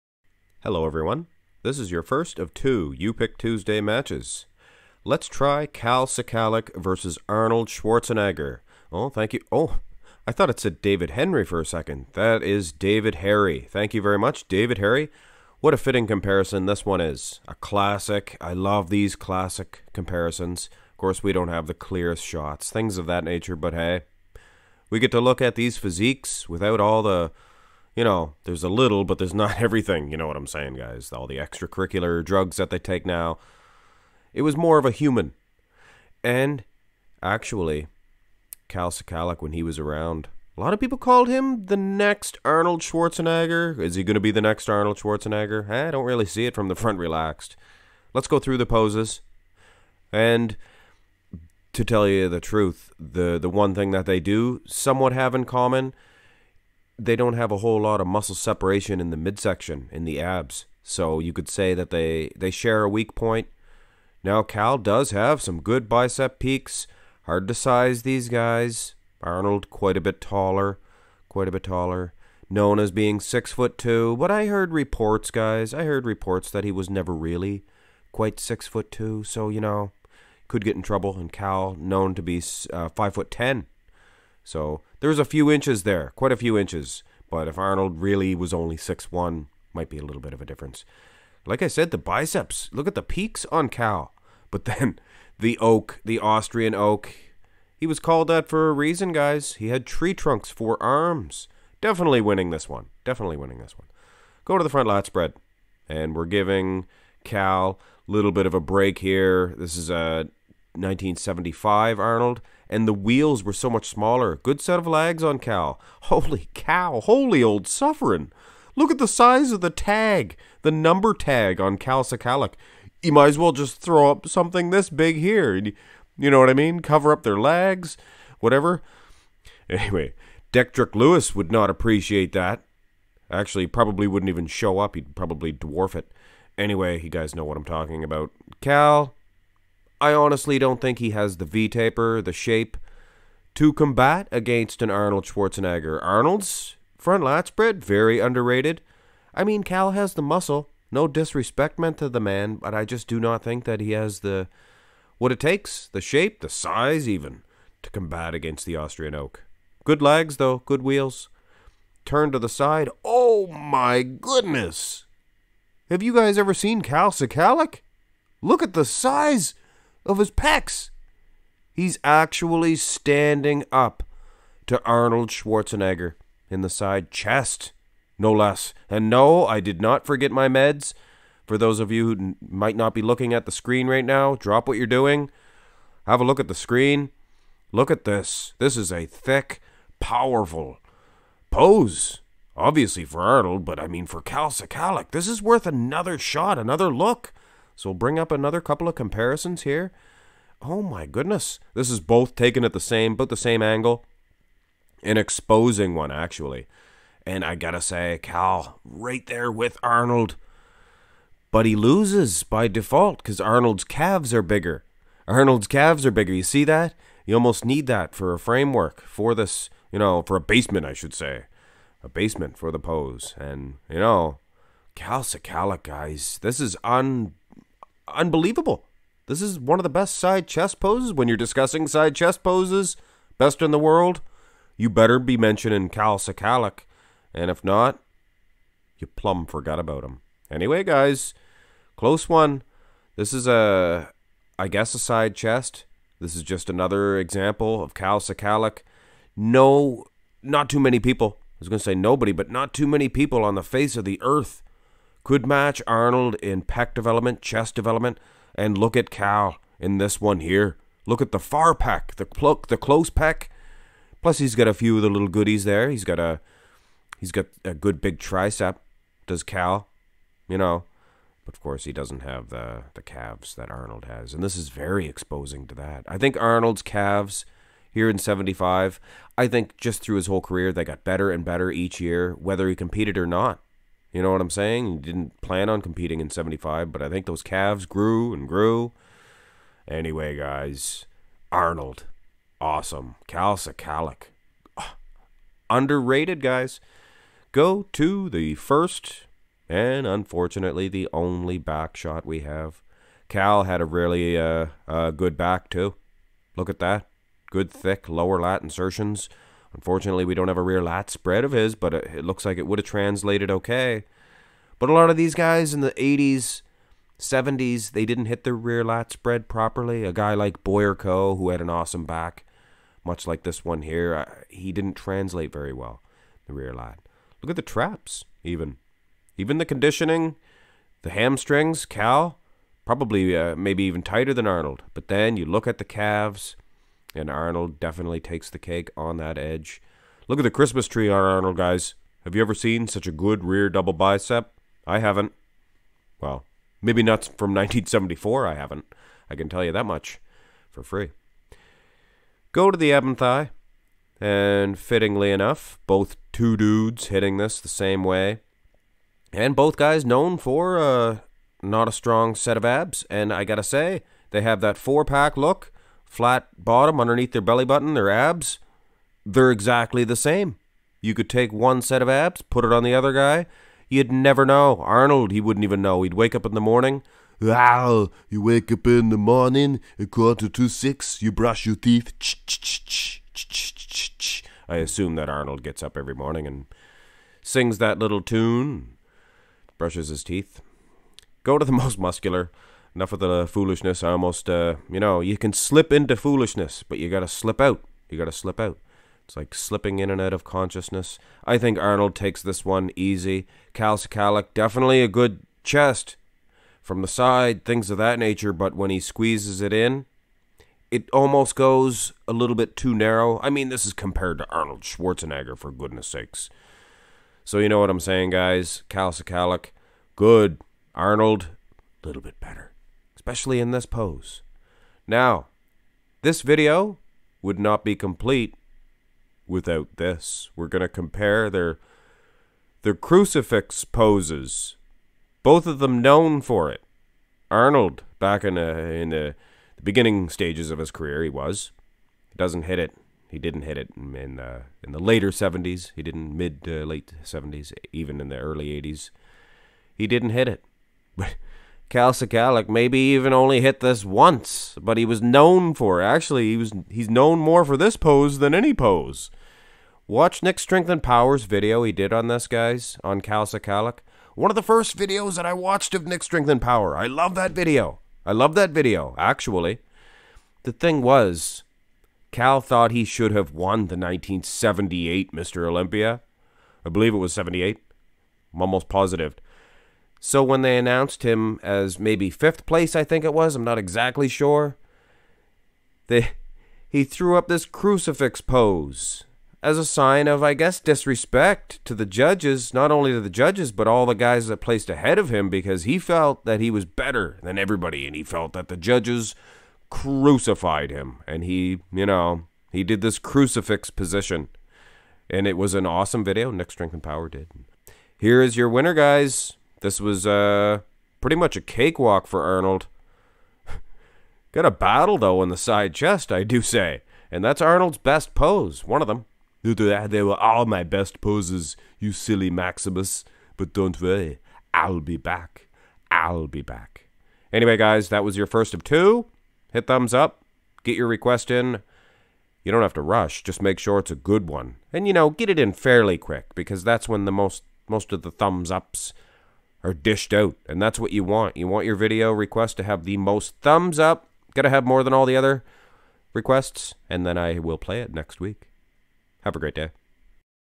Kalman Szkalak vs Arnold Schwarzenegger
This is a fan requested comparison between Kalman Szkalak and Arnold Schwarzenegger.